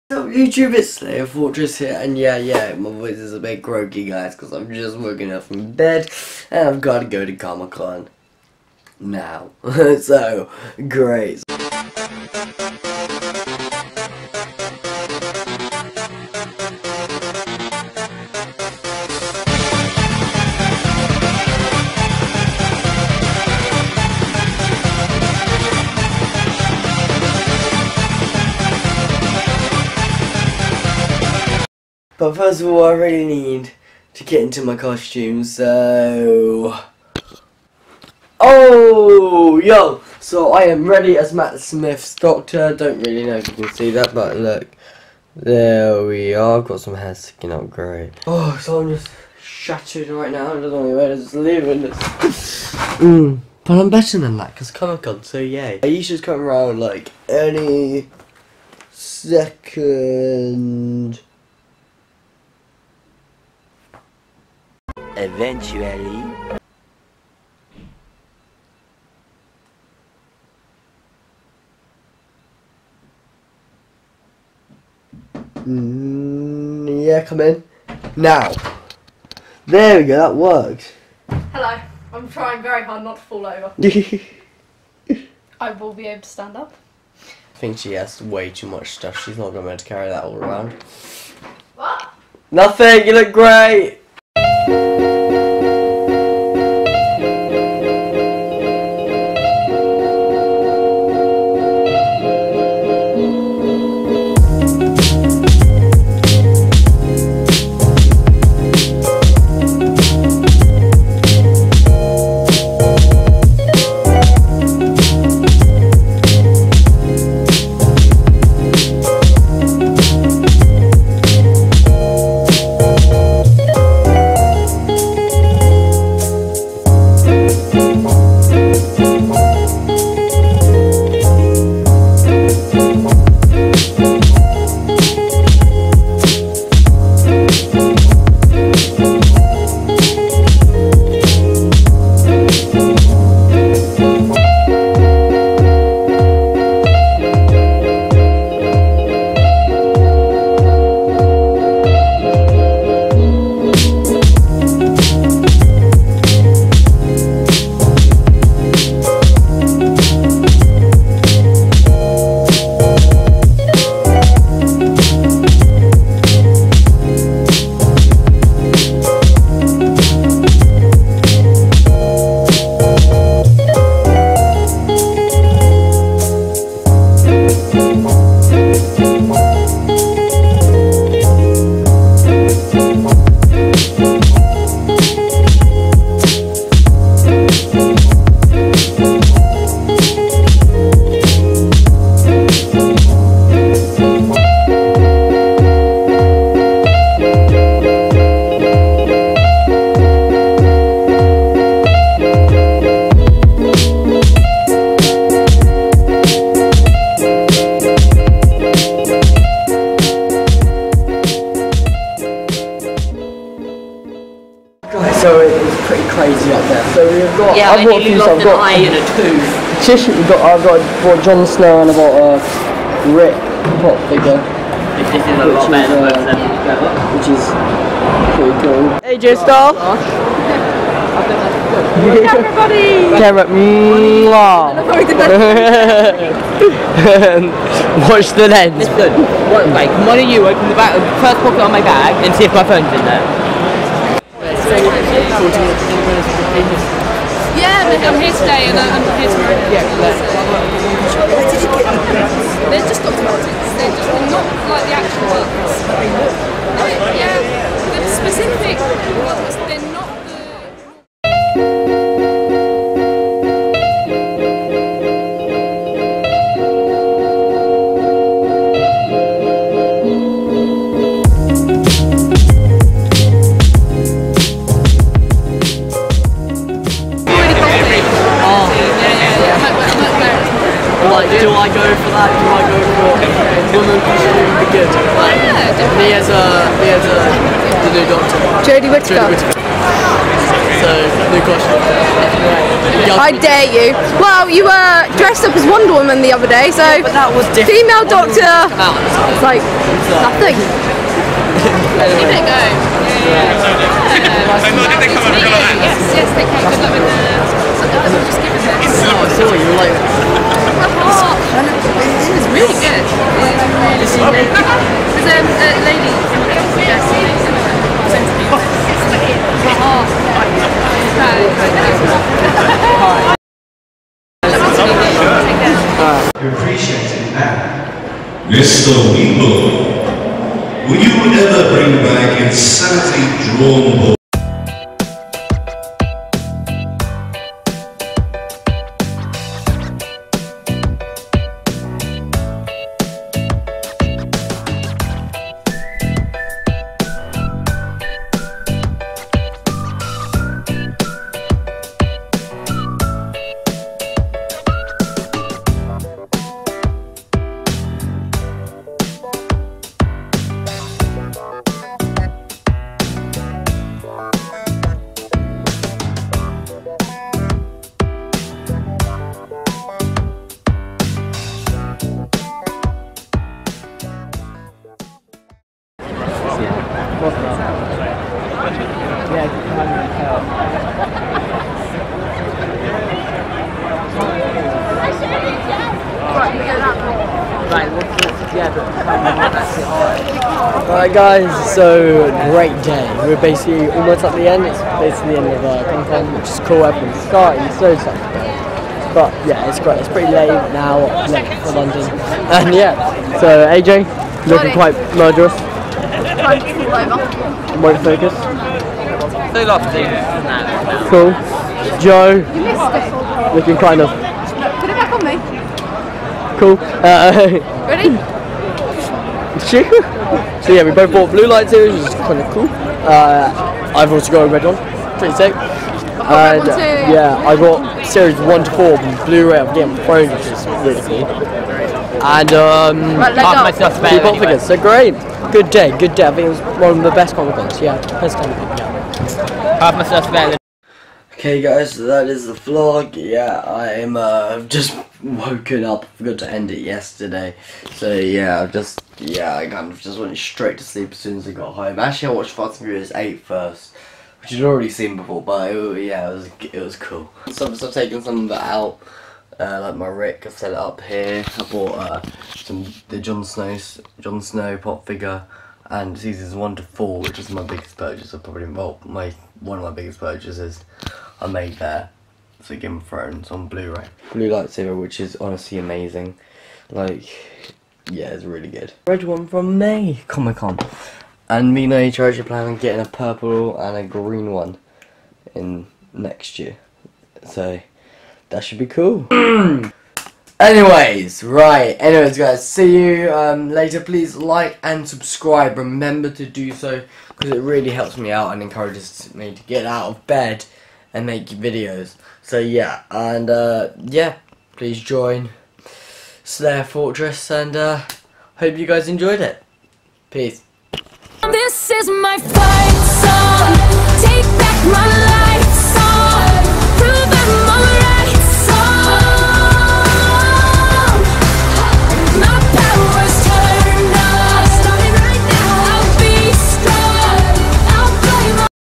Meeting Christel from Doctor Who channel!! | London MCM October comic con | vlog 2017
Thanks guys for watching like and sub and i will see you in my next video.